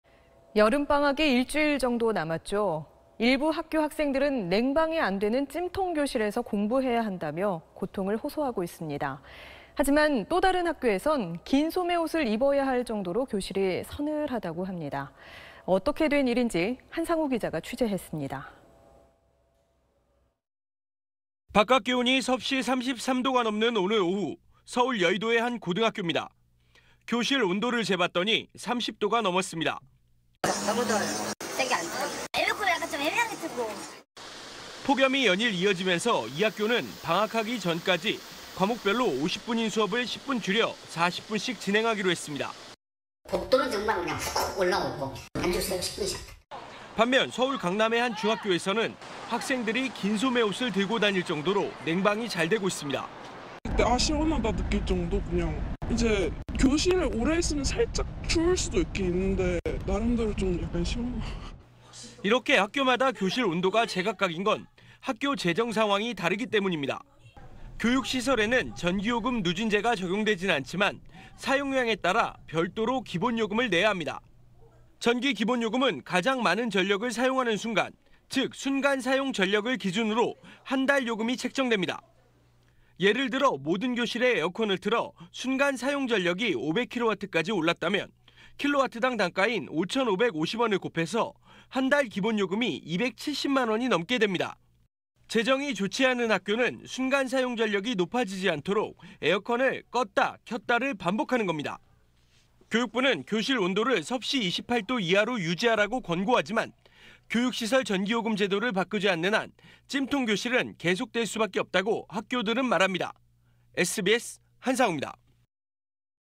"더워요""추워요"…학교마다 제각각인 '교실 온도', 이유는 / SBS
여름 방학이 일주일 정도 남았죠. 일부 학교 학생들은 냉방이 안 되는 찜통 교실에서 공부해야 한다며 고통을 호소하고 있습니다. 하지만 또 다른 학교에서는 긴 소매 옷을 입어야 할 정도로 교실이 서늘하다고 합니다.